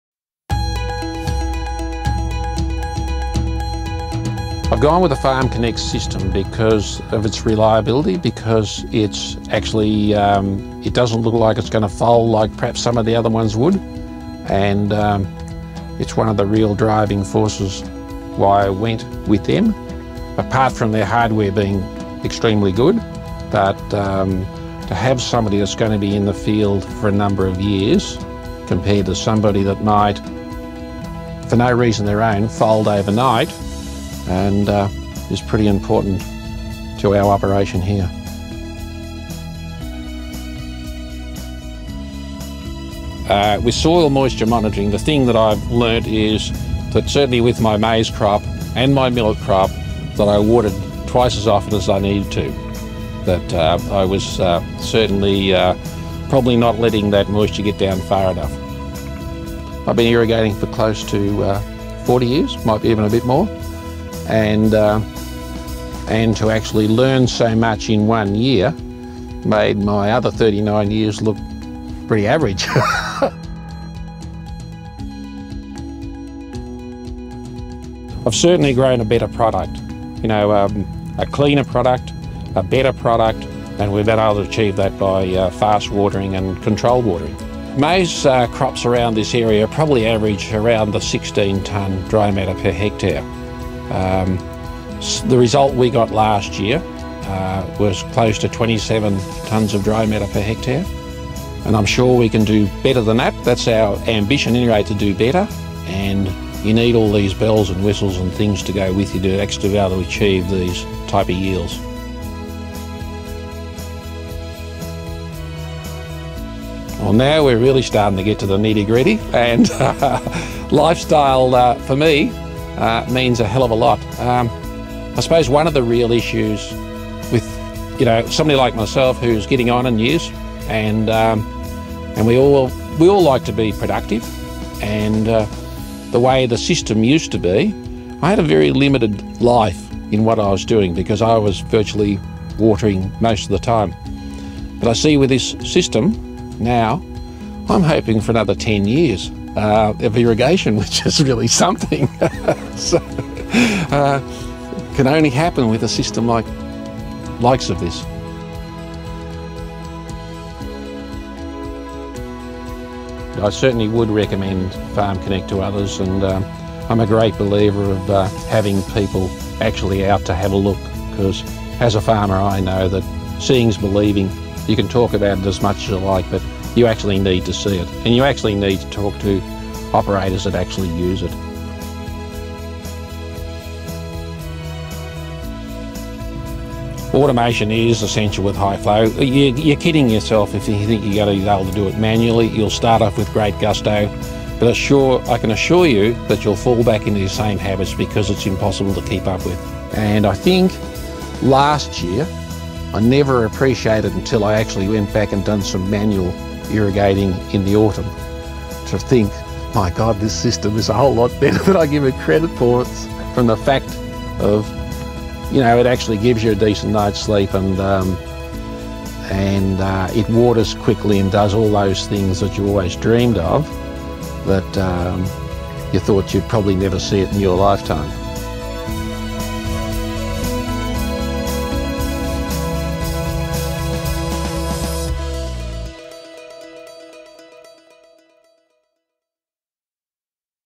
How does FarmConnect benefit Russell Pell?
With FarmConnect®, farmers can grow higher-value crops using less labour, water and energy. FarmConnect is Rubicon Water's suite of products and solutions for farmers. FarmConnect's modular hardware and software enables precisely-timed application of water to crops using high-flow, precision surface irrigation.